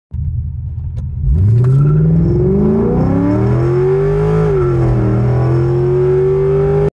2008 Corvette C6 LS3 0-60 acceleration
Billy Boat Exhaust
K&N Cold Air Intake
Long tube Headers
400 whp